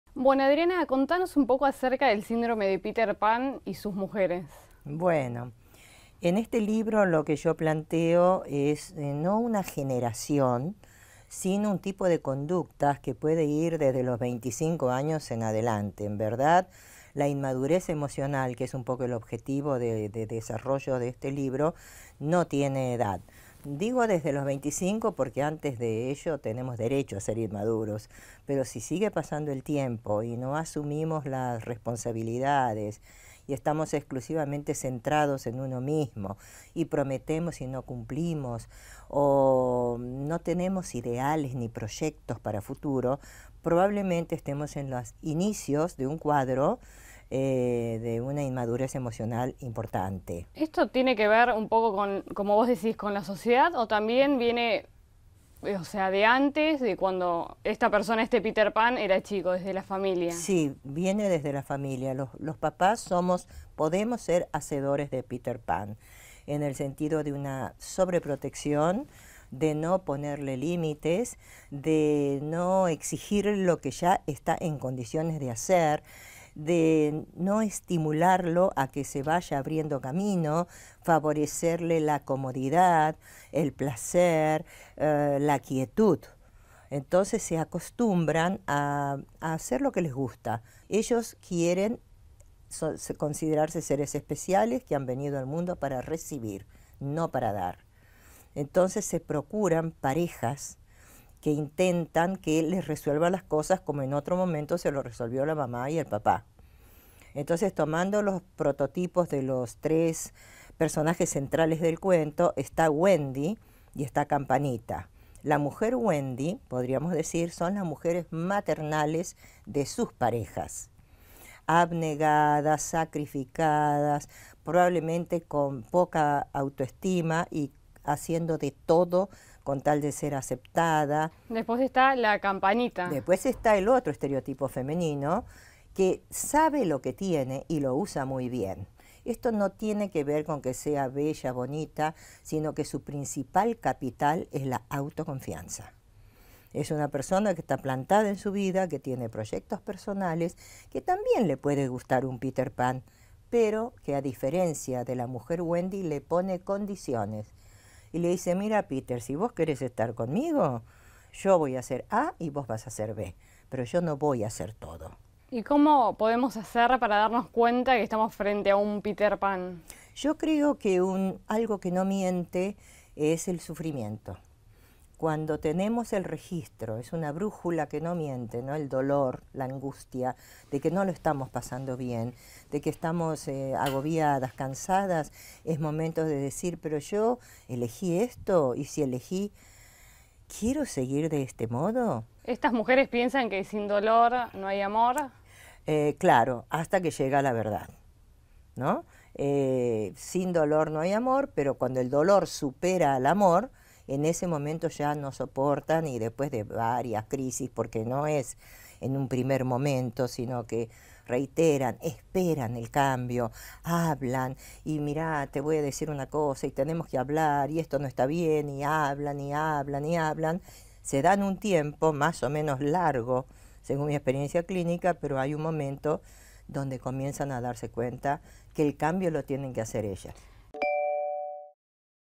Síndrome de Peter Pan: el drama de ser un "niño eterno"
En qué consiste el trastorno de personalidad de aquellos que nunca pueden abandonar la fase juvenil a lo largo de toda su vida adulta. Una especialista analizó para Infobae las claves para poder lidiar con el problema